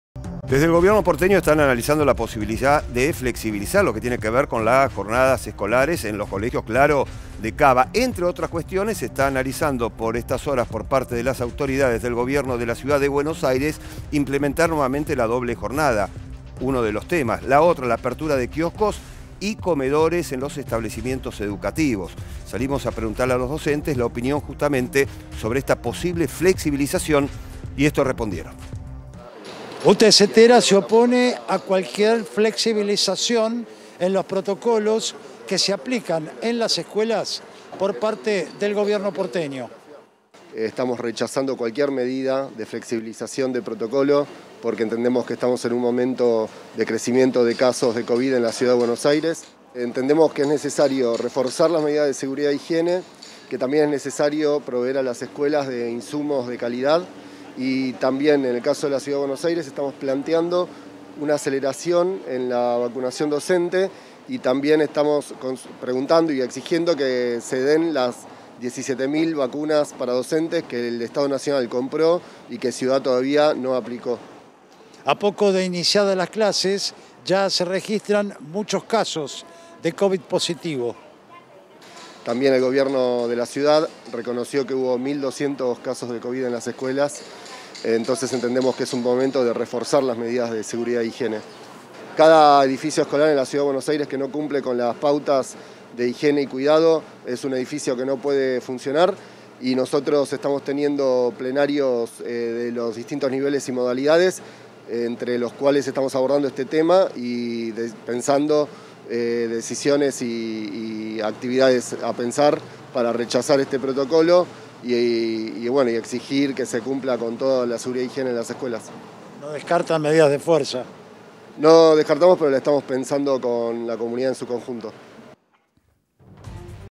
Docentes rechazan la flexibilización del protocolo porteño
Dialogamos con Pablo Francisco, de UTE-CETERA, quien afirmó que desde el gremio están "rechazando cualquier medida de flexibilización del protocolo, porque estamos en un momento de crecimiento de los casos de covid en la Ciudad de Buenos Aires". Además, exigen la urgente vacunación de las y los docentes porteños.